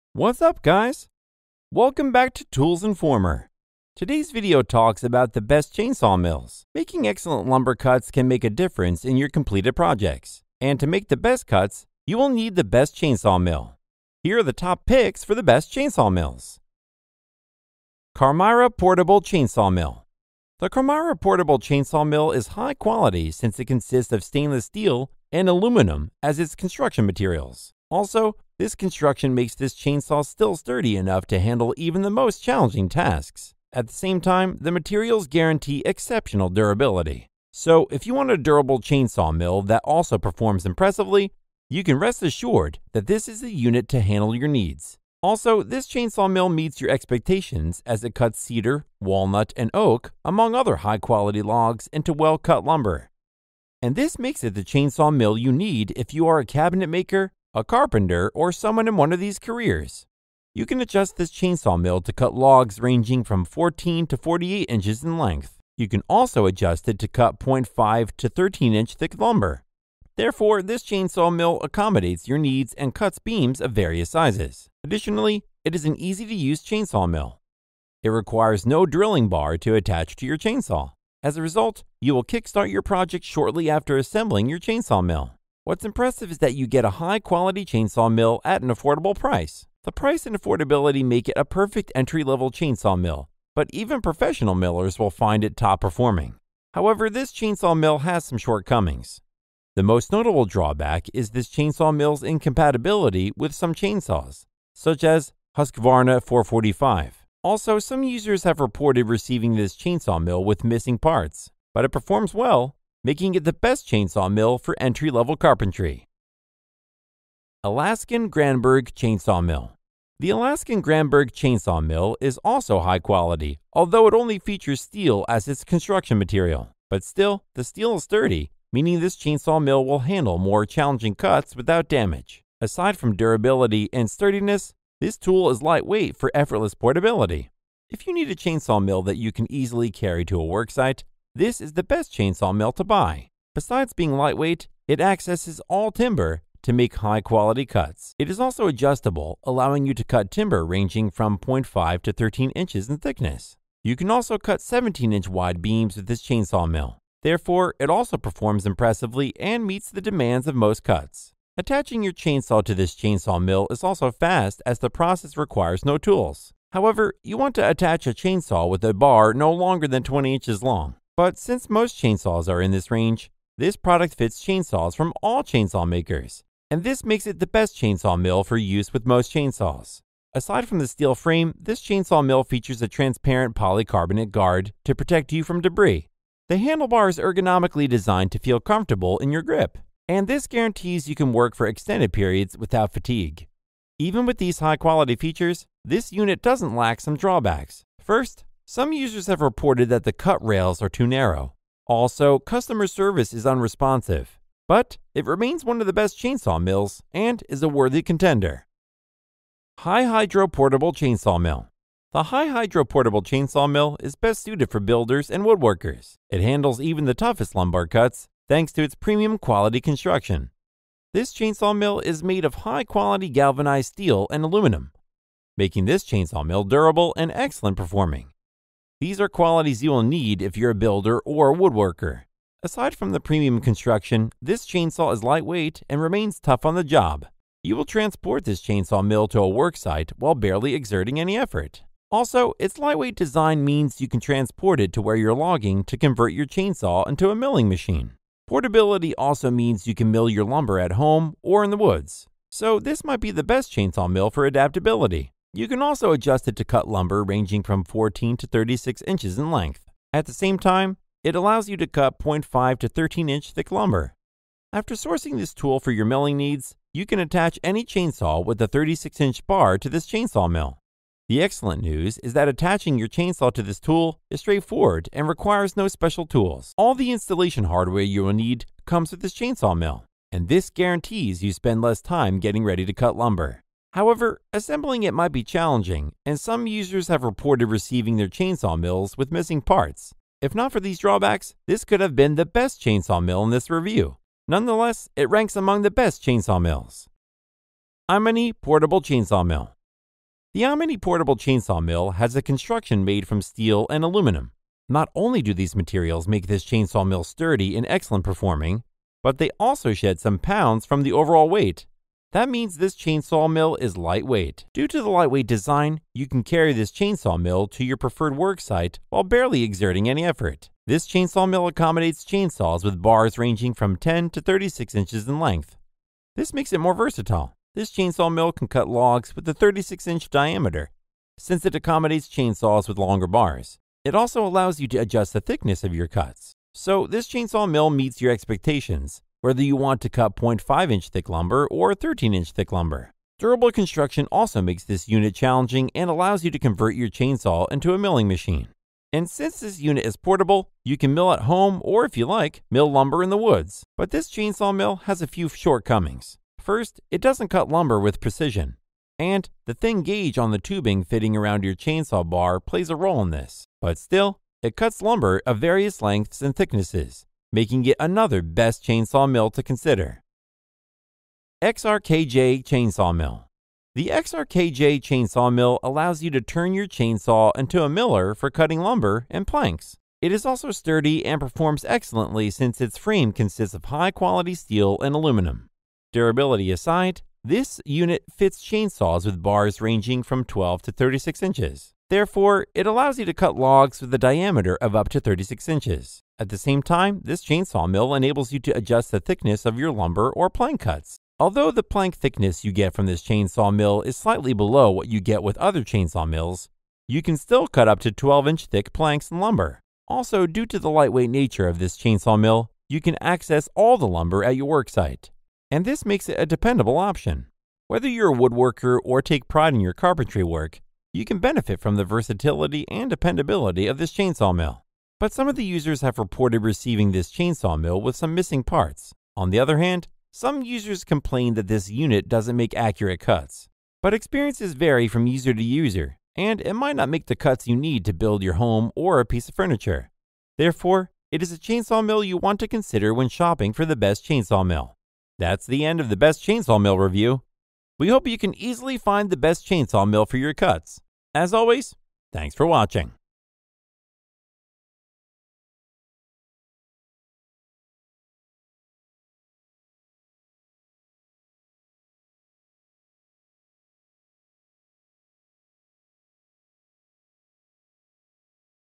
Best Chainsaw Mills 2024 - Top 5 Picks
►Purchase Link ✅

►TIME STAMP◄
00:00  INTRO.
00:18  Carmyra Portable Chainsaw Mill Review.
01:57  ALASKAN Granberg Chainsaw Mill Review.
03:30  HiHydro Portable Chainsaw Mill Review.
04:59  Imony Portable Chainsaw Mill Review.
06:11  XRKJ Chainsaw Mill Review.
07:24  OUTRO.